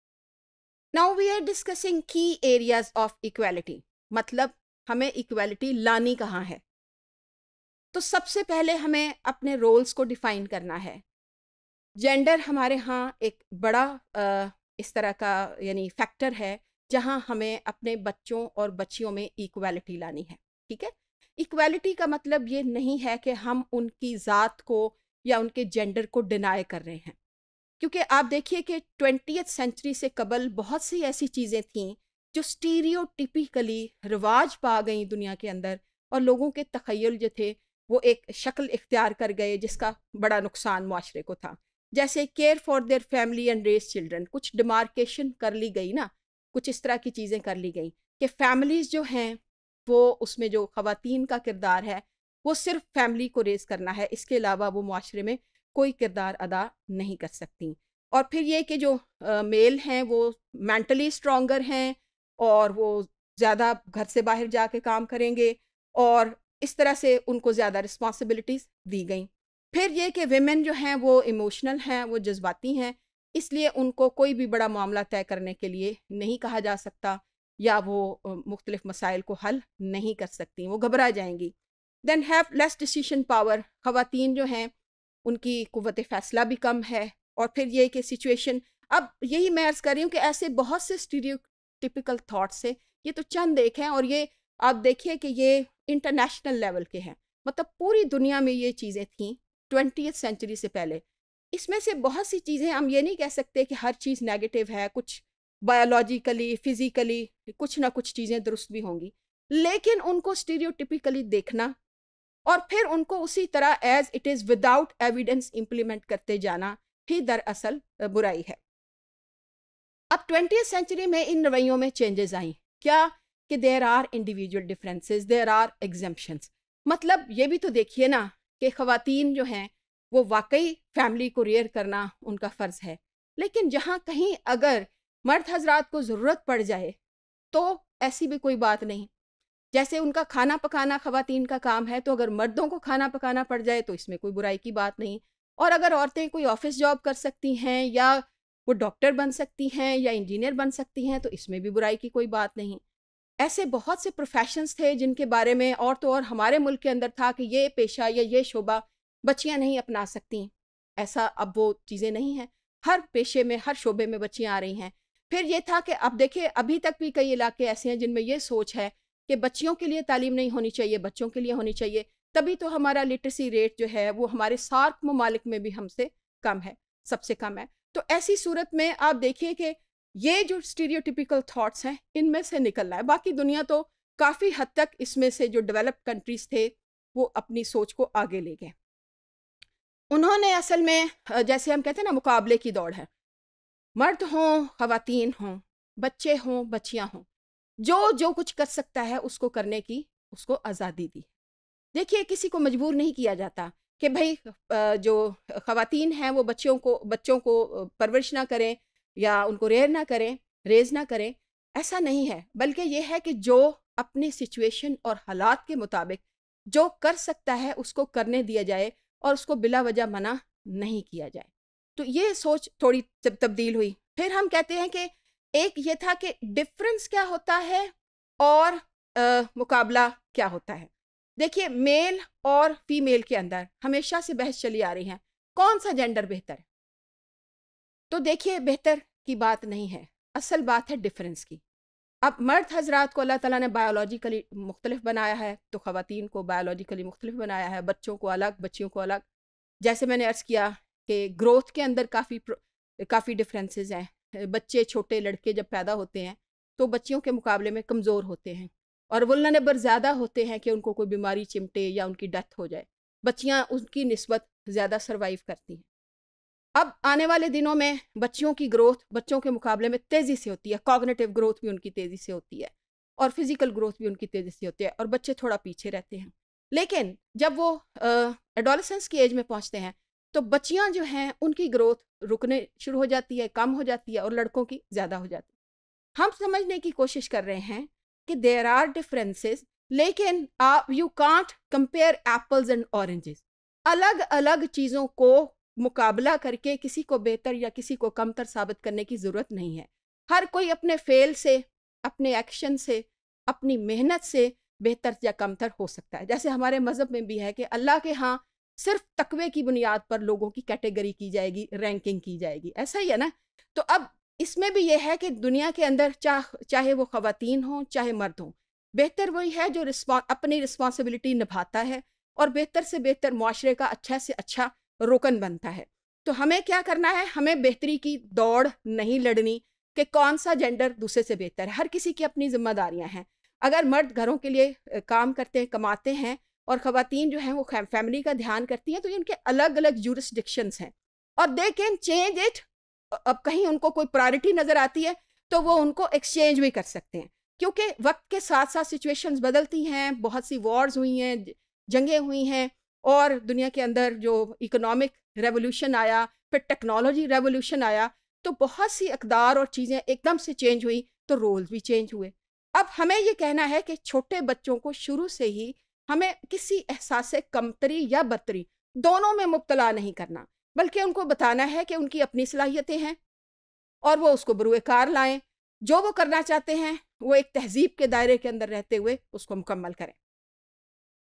Key Areas of Equality | Trends and Issues in Early Childhood Education | ECE402_Topic148
Topic148: Key Areas of Equality
By Dr. Afifa Khanam